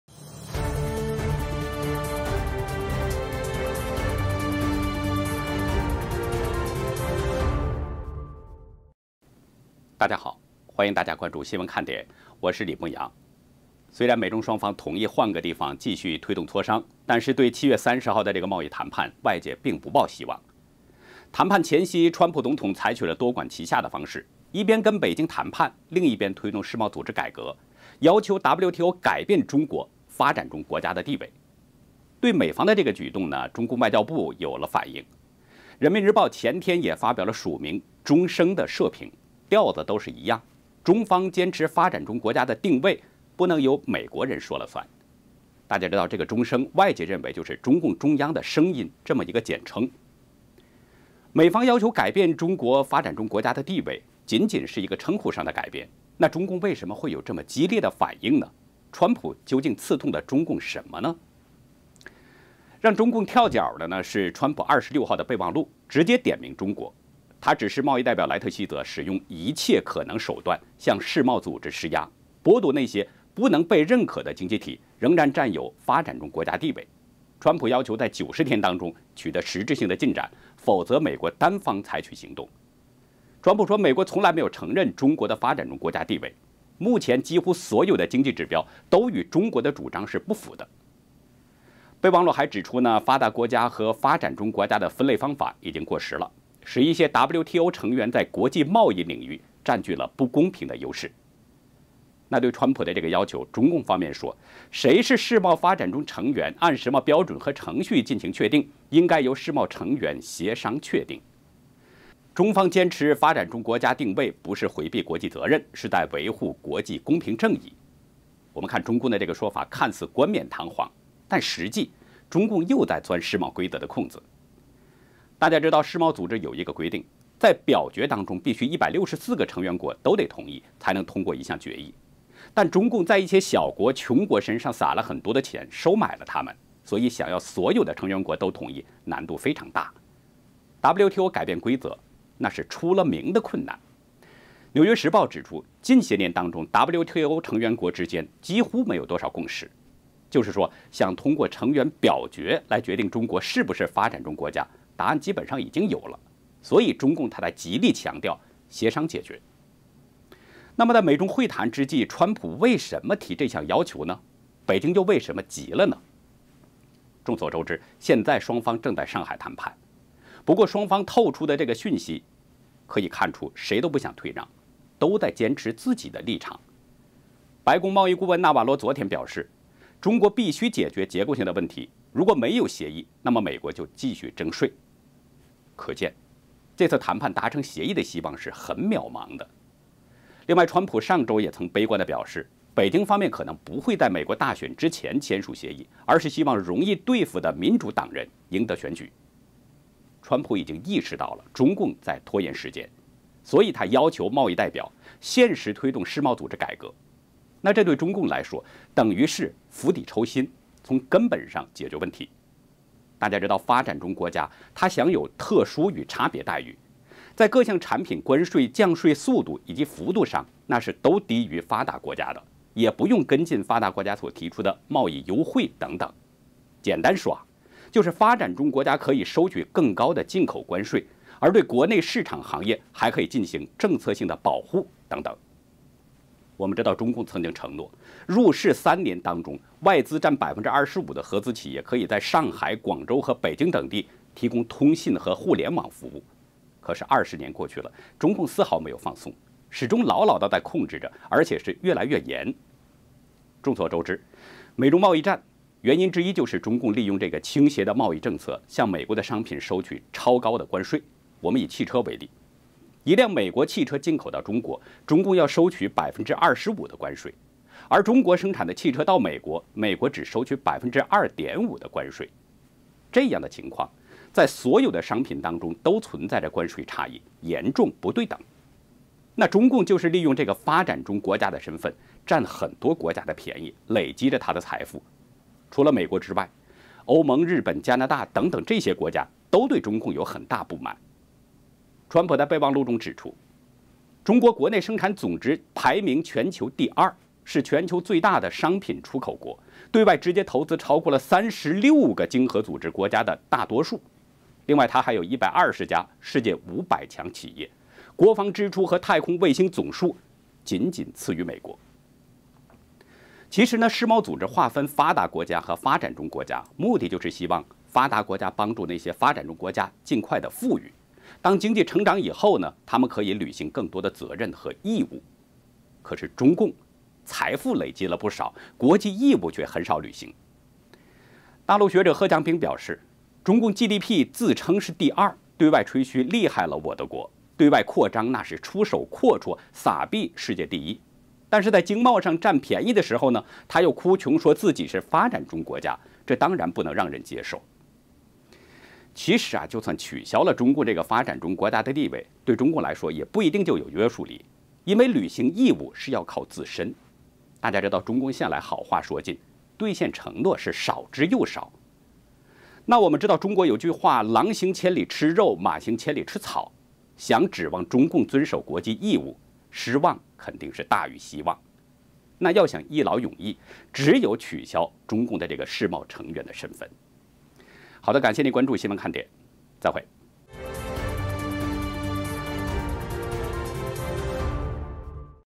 川普只給90天時限，從根子上解決問題，北京「跳腳」了【新聞看點】（2019/07/30）
雖然美中雙方同意換個地方，試圖推動磋商。但是對今天（7月30日）的美中#貿易談判 ，外界並不抱希望。談判前夕，美國總統#川普 採取了多管齊下的方式。一邊繼續與北京方面談判，另一邊推動世貿組織（WTO）改革，要求WTO改變中國「發展中國家 」的地位。
對美方的這個舉動，中共外交部發言人華春瑩在昨天例行記者會上作了回應，中共黨媒人民日報前天也發表署名「鐘聲」的社評。調子都是一樣，中方堅持發展中國家的定位，不能由美國人說了算。「鐘聲」，外界認為是「中共中央的聲音」的簡稱。
美方要求改變中國發展中國家地位，僅僅是稱呼上的改變，中共為什麼會有激烈的反應？川普究竟刺痛了中共什麼？